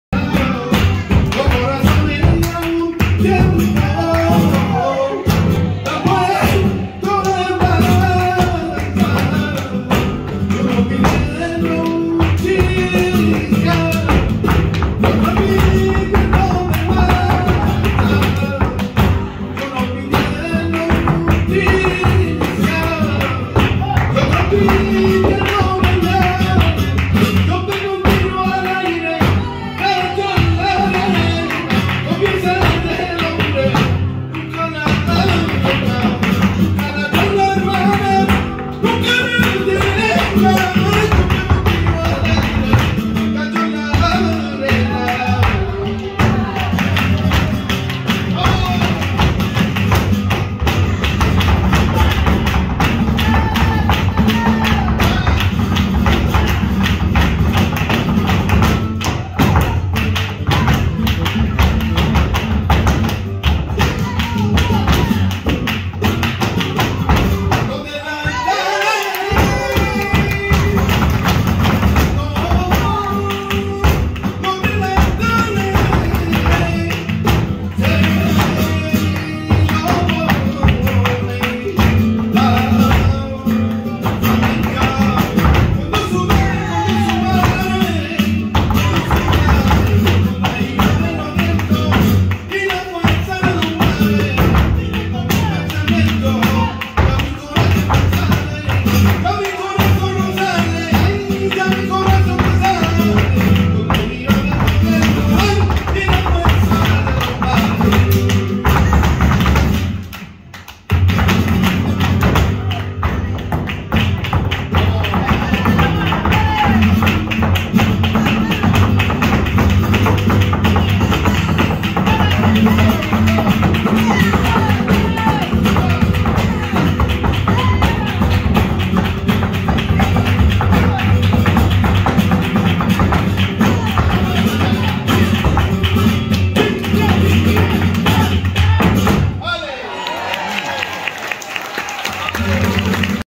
Alegrías - FLAMENCO @TheSoundRoom - 10/28
Flamenco at the Sound Room w/ Agua Clara Flamenco

David McLean - La Guitarra
Pirouz Ebadypour "de Caspio" - Cante
Clara Rodriguez - Baile
Mizuho Sato - Baile
Damien Alvarez- Baile